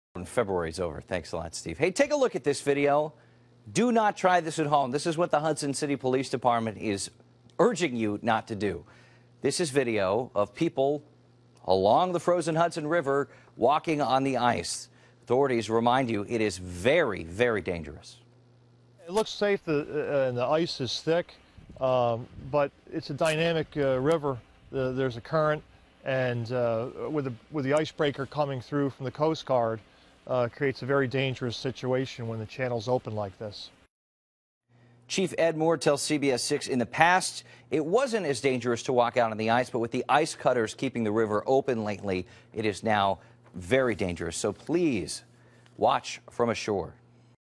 Stay off the ice!
HUDSON -- It may look solid, but Hudson Police are warning people to stay off the frozen Hudson River.
Why? Because ice cutters are keeping the river open and unstable which means it could crack and a person could easily fall into the river.
Watch the report above showing surveillance video of what police are saying NOT to do.